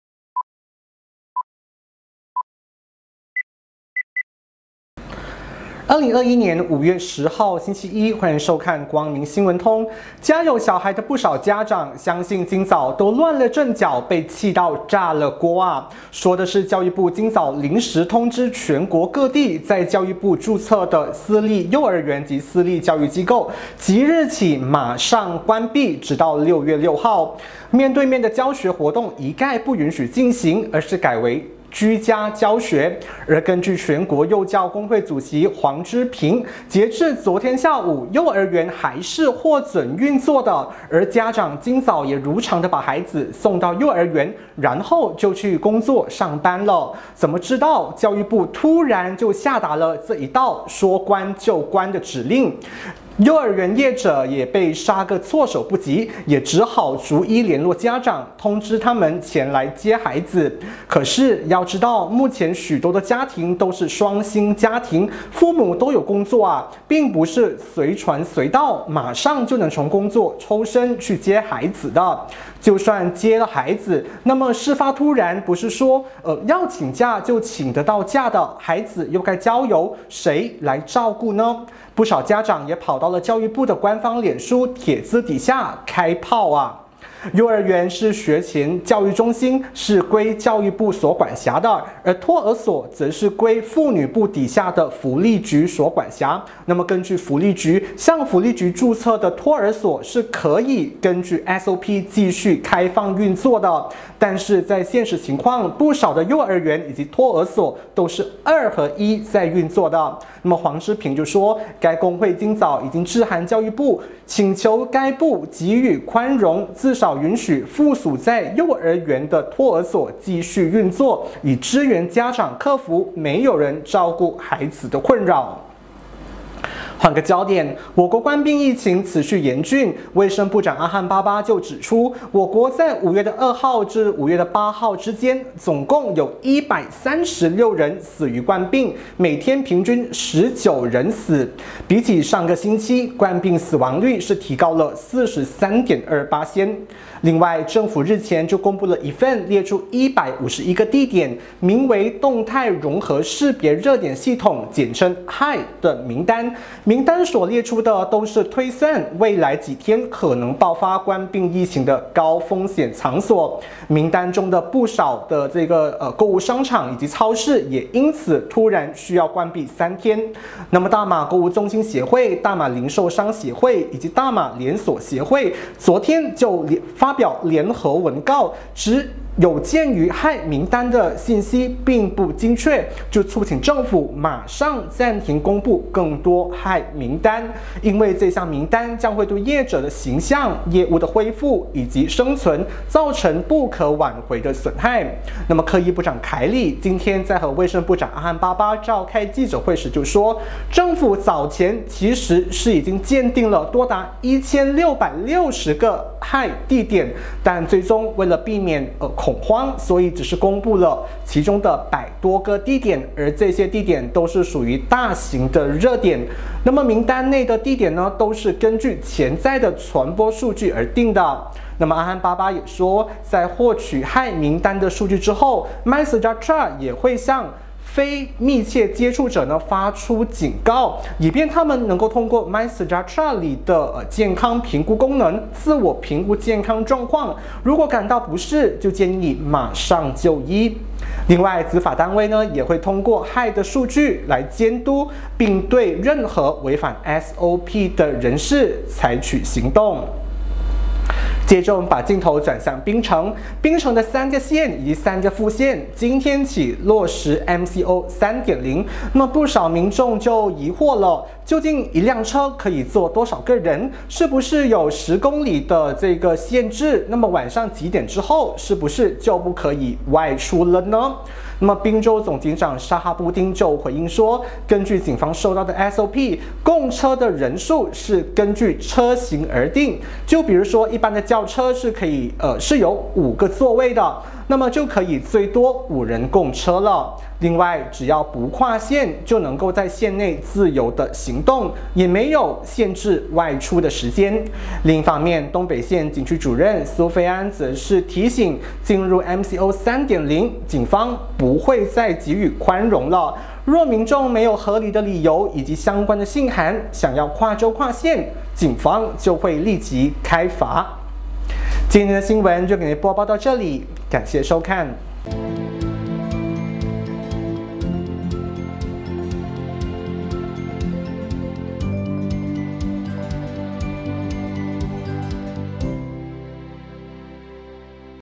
【光明新聞通】2021年5月11日夜報封面焦點
◆私立幼兒園教育機構 即起關閉至6月6日
◆冠病死亡率增 第18週每天19死
◆凱里：避免恐慌 僅公佈大型熱點
◆沒限時外出 共車人數看車型
光明新聞通，讓您掌握新聞時事，一點就通！與你一同關注11/5/2020光明夜報焦點。欲知詳細新聞內容，購閱《光明日報》，點擊 或訂閱光明電子報